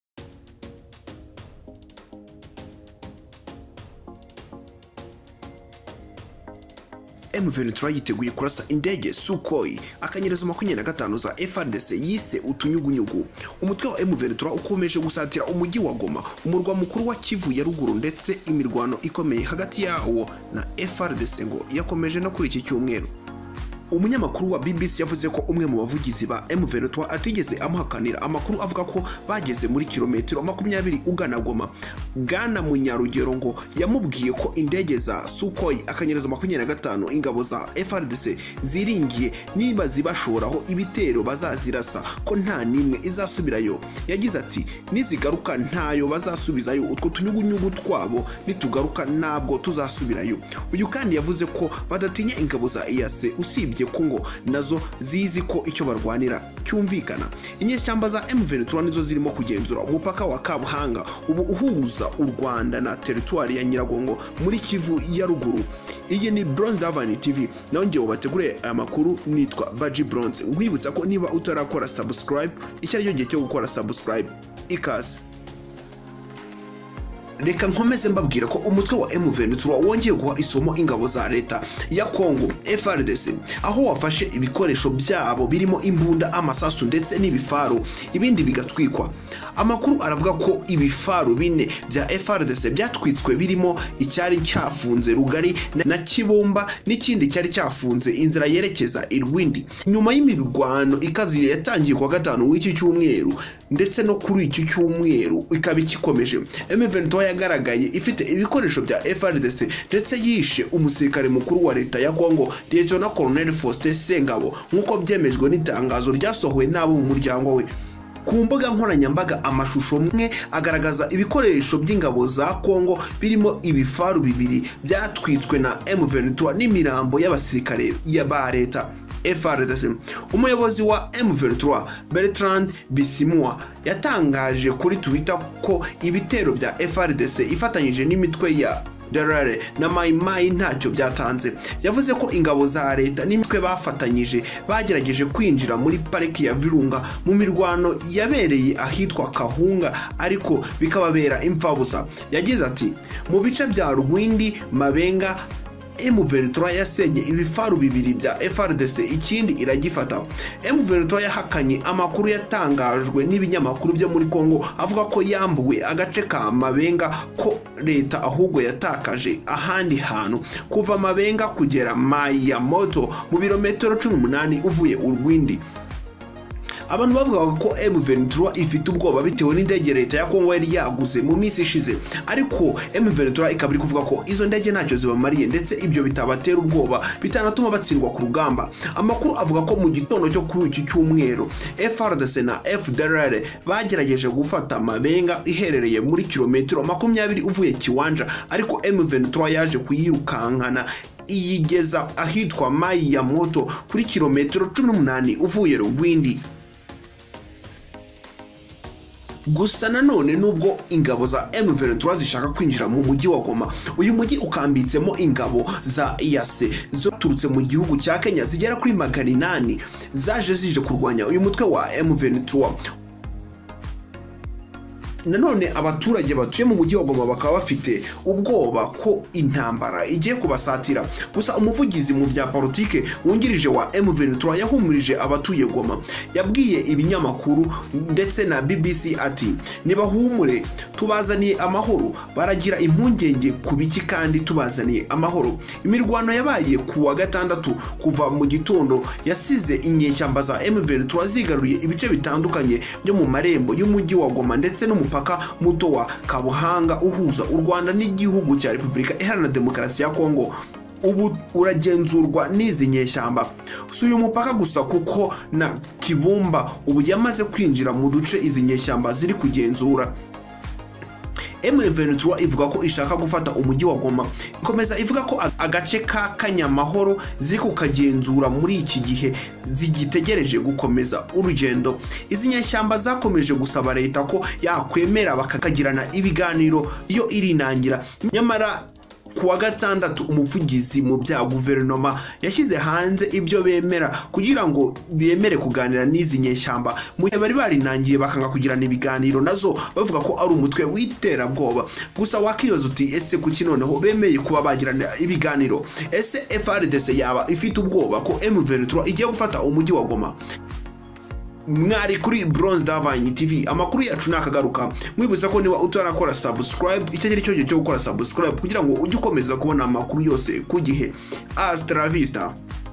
RDC UBWOBA BWAYITASHYE BEMEYE IBIGANIRO🚨 M23 YABAMBUYE IBIFARU 4//M23 YITEGUYE KURASA"SUKHOI-25
M23 YIYEMEJE KURASA INDEGE "SUKHOI-25" ZA FRDC YISE "UTUNYUGUNYUGU" HAGATI AHO RDC YEMEYE IBIGANIRO// M23 MU 20 KM KUGIRANGO BAGERE GOMA// BYOSE NI KURI BRONZEDIVINE TV